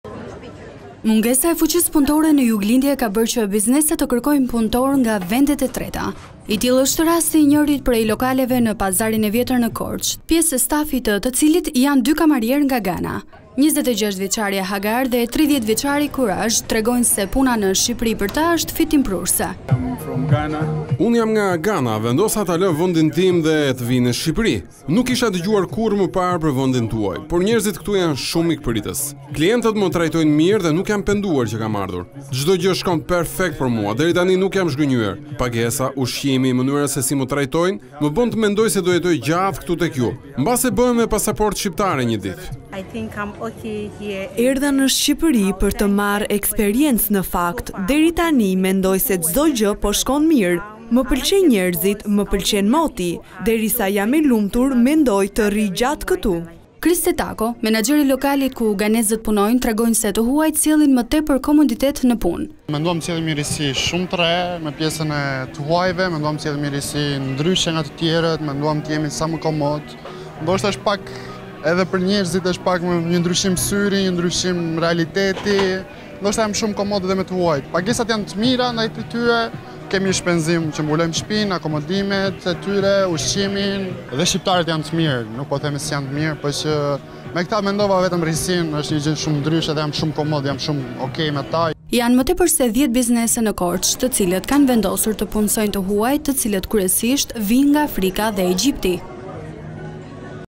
Të huajt punësohen në Korçë, ganezët shërbejnë si kamarierë në bare e restorante - Vizion Plus
Të huajt punësohen në Korçë, ganezët shërbejnë si kamarierë në bare e restorante.